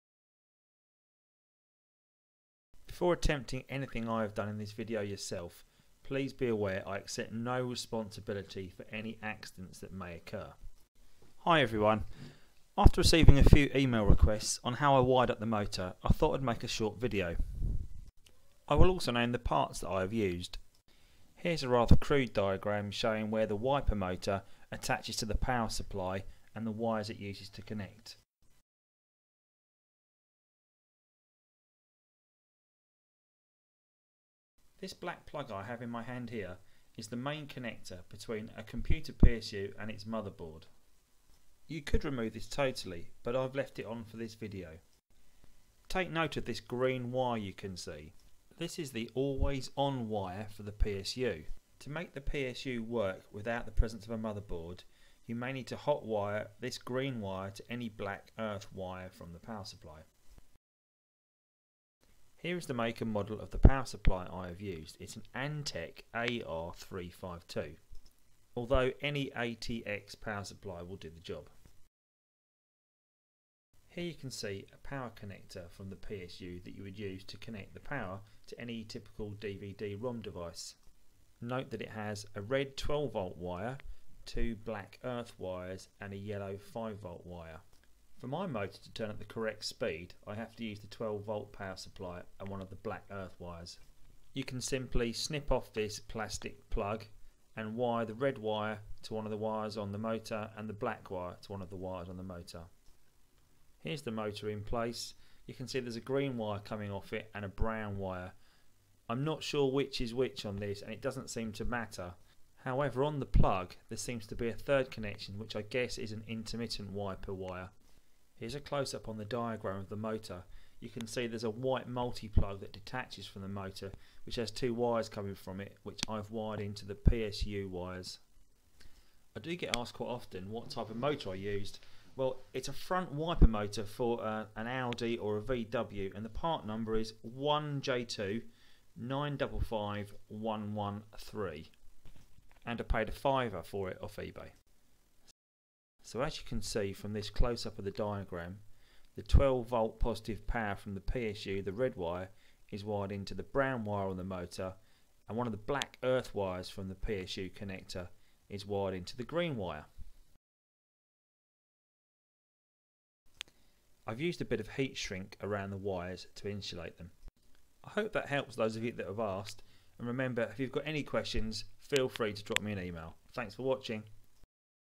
How to build a coin pusher - Part 6. Wiring a wiper motor to a PSU
Although my coin pusher is finished, I have been asked how the wiper motor is wired to the power supply. If you want to see see how my pusher mechanism is wired, watch on! Part of my build a penny pusher series!

Please do go and check out my coin pusher "push" mechanism to understand how the motor works and how the coin shelf moves!